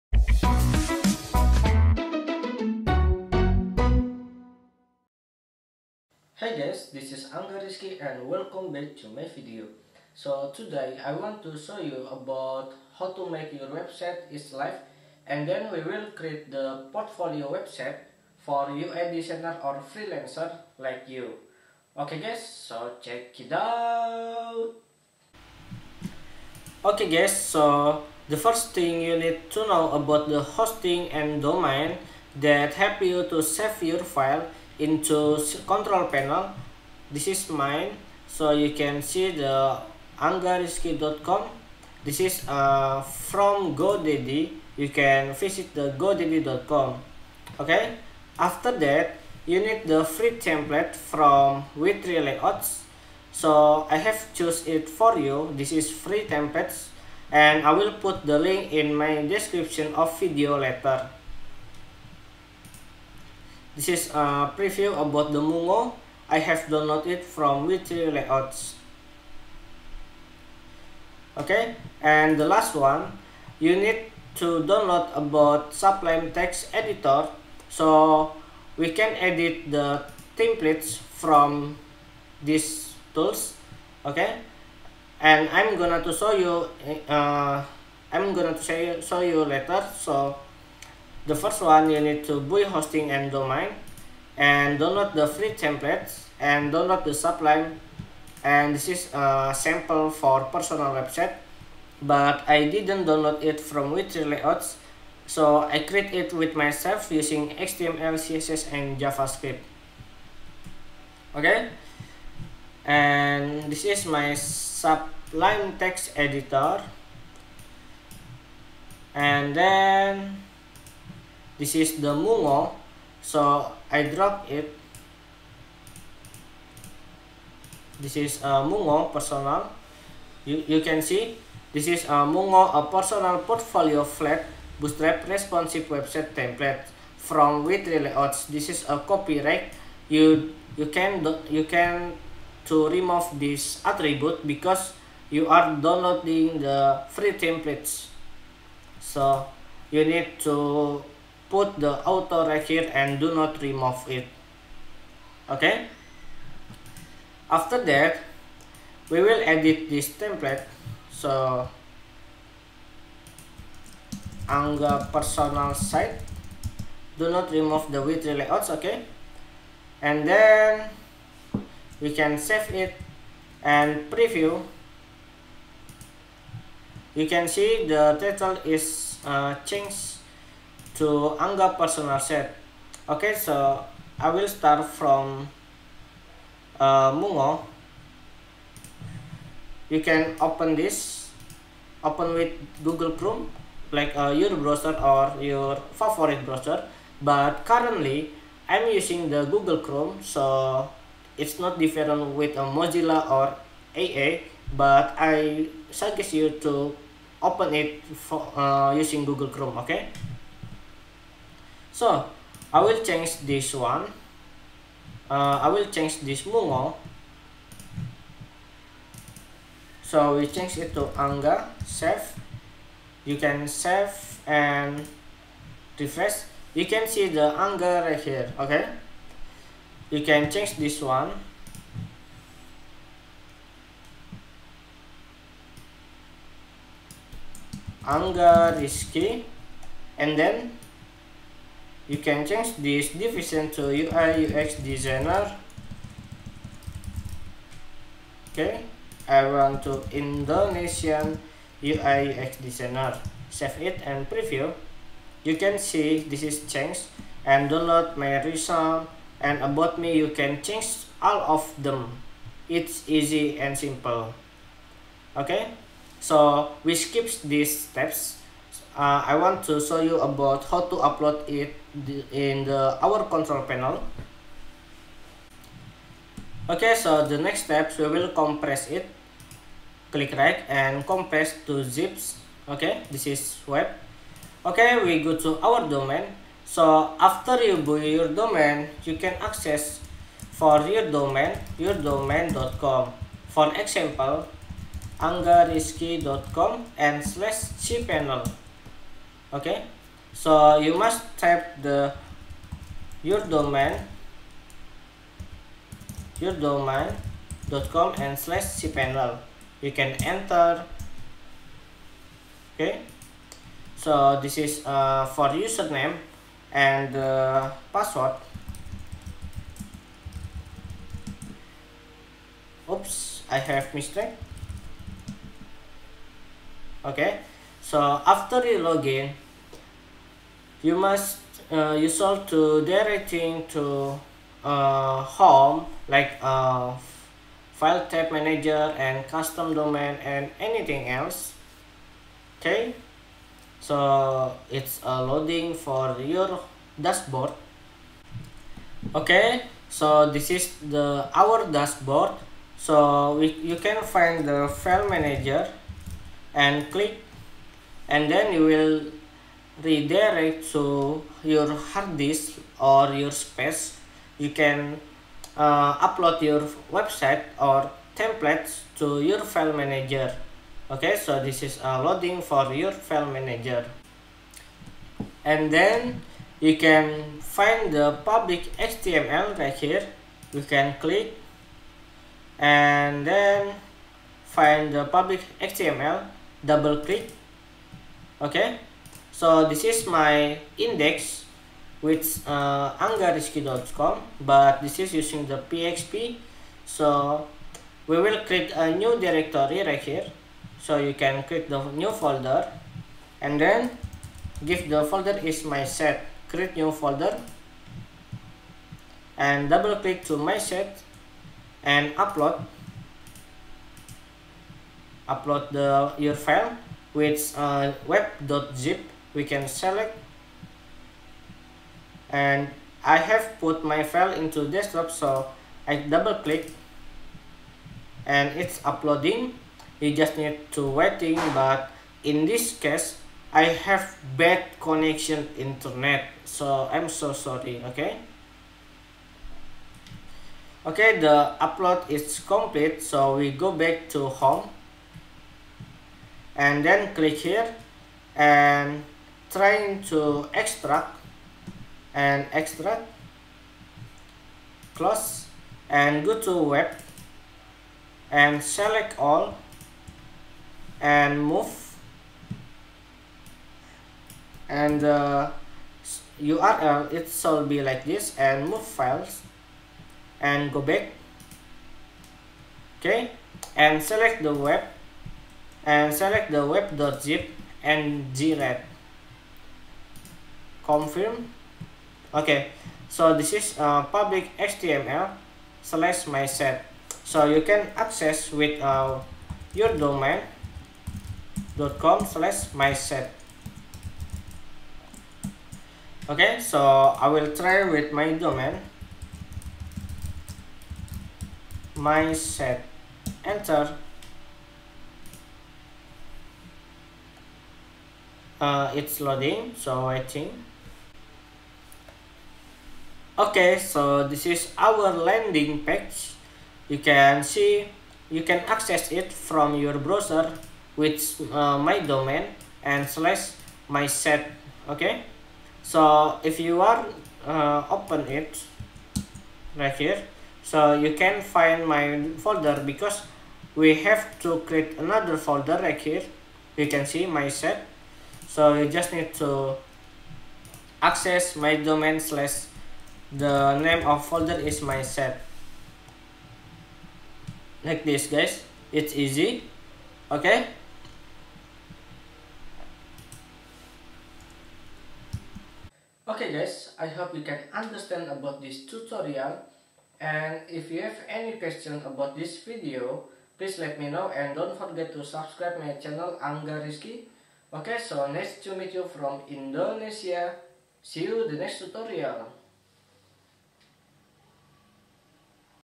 How to Make Your Personal Site is Live
How to Make Your Personal Site is Live, it's good for freelancer and designer or programmer. Build your site to show your work or portfolio.

1. It's free (you just need the google mail account)
2. Up to date (every my videos in your mail)
3. Help me to grow up this channel

Learn Android with my friend:
1. Robby Dianputra: